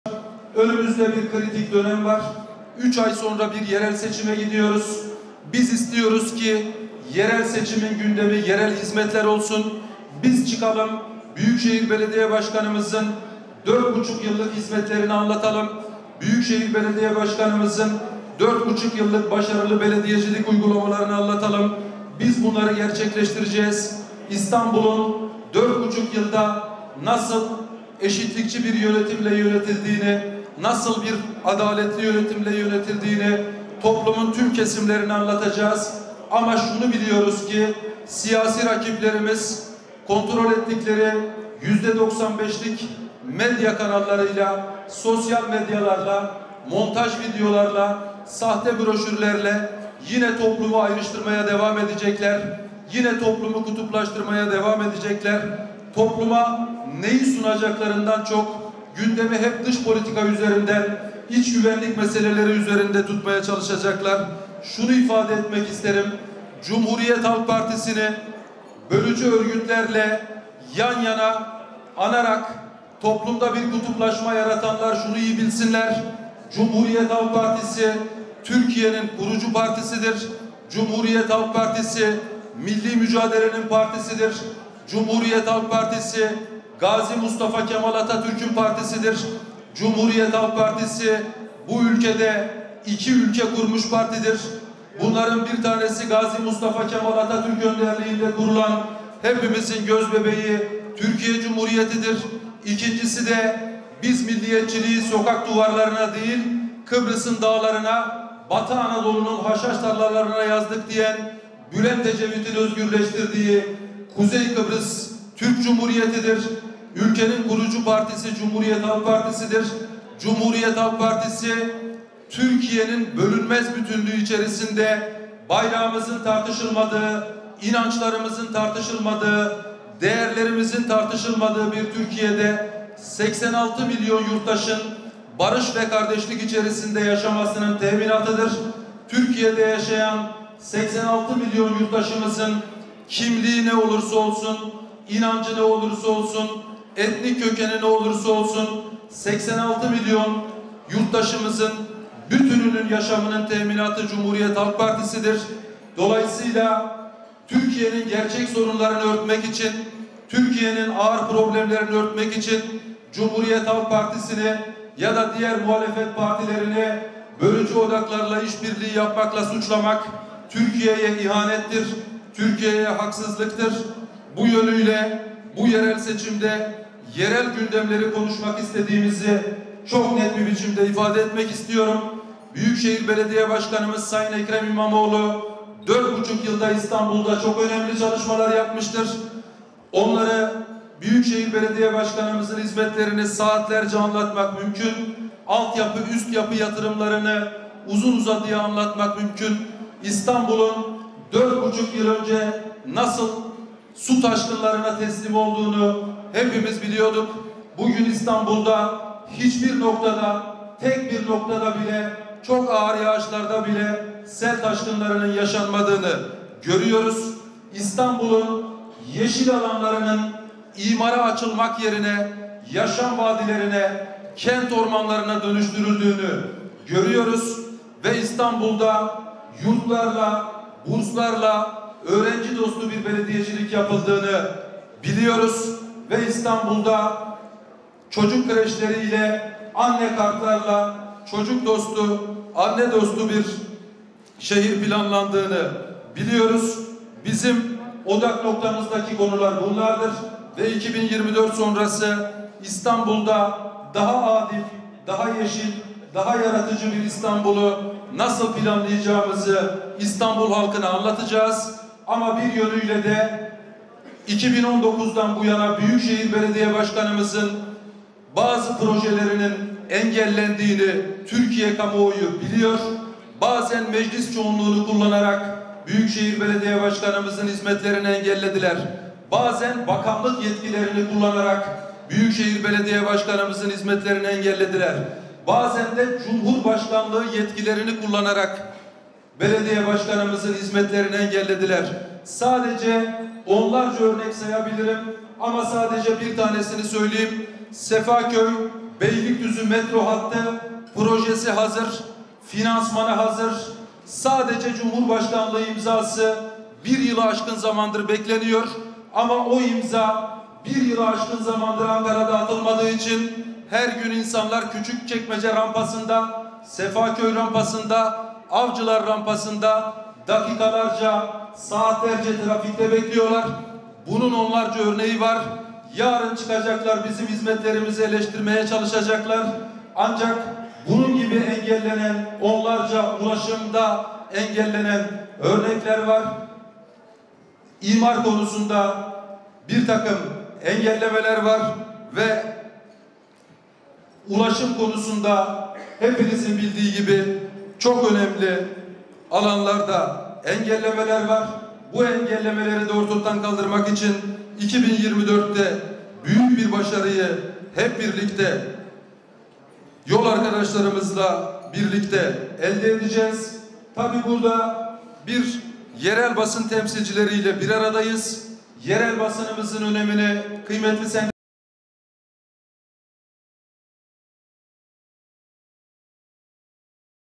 CHP'li Özgür Çelik'ten, 'bölücülük' suçlamalarına sert yanıt: İhanettir, haksızlıktır...
CHP İstanbul İl Başkanı Özgür Çelik, 10 Ocak Gazeteciler Günü nedeniyle Zeytinburnu’ndaki 1453 Çırpıcı Sosyal Tesisleri’nde bugün yerel basınla bir araya geldi. Burada gazetecilere hitap eden Çelik, günün anlamlı ancak ocak ayının gazeteciler ve gazetecilik açısından kapkara bir ay olduğunu da dile getirdi. Çelik, şunları söyledi:

Ülkenin kurucu partisi CHP’dir. CHP, Türkiye’nin bölünmez bütünlüğü içerisinde bayrağımızın, inançlarımızın, değerlerimizin tartışılmadığı bir Türkiye’de 86 milyon yurttaşın barış ve kardeşlik içerisinde yaşamasının teminatıdır. Türkiye’de yaşayan 86 milyon yurttaşımızın kimliği, inancı, kökeni ne olursa olsun bütününün yaşamının teminatı CHP’dir. Bu yönüyle bu yerel seçimde yerel gündemleri konuşmak istediğimizi çok net bir biçimde ifade etmek istiyorum. Büyükşehir Belediye Başkanımız Sayın Ekrem İmamoğlu, 4,5 yılda İstanbul’da çok önemli çalışmalar yapmıştır. Onları, hizmetleri saatlerce anlatmak mümkün. Altyapı, üstyapı yatırımlarını uzun uzadıya anlatmak mümkün. İstanbul’un 4,5 yıl önce nasıl su taşkınlarına teslim olduğunu hepimiz biliyorduk. Bugün İstanbul’da tek bir noktada bile, çok ağır yağışlarda bile sel taşkınlarının yaşanmadığını görüyoruz.

Bugün Türkiye çok ağır koşullar yaşıyor. Bugün Türkiye’de çok ciddi bir ekonomik kriz var. Bugün Türkiye’de çok ciddi bir geçim sıkıntısı var. Bugün Türkiye’nin deprem başta olmak üzere çok önemli problemleri ancak ne yazık ki medya kanallarının yüzde 95’inde Türkiye’nin ekonomik sorunlarını izleyemiyoruz, okuyamıyoruz. Türkiye’nin kentsel dönüşüm sorunlarını izleyemiyoruz, okuyamıyoruz. Bugün ne yazık ki sadece dış politika ve güvenlik konusunda bir hamaset siyasetiyle medya, toplumu yönlendirme çabası içerisine gidiyor. Aynı zamanda bunu sadece ana akım medya yoluyla gerçekleştirmiyorlar. Önümüzde bir kritik dönem var. Üç ay sonra bir yerel seçime gidiyoruz. Biz istiyoruz ki, yerel seçimin gündemi yerel hizmetler olsun. Biz çıkalım, Büyükşehir Belediye Başkanımızın 4,5 yıllık hizmetlerini, başarılı belediyecilik uygulamalarını anlatalım. Biz bunları gerçekleştireceğiz.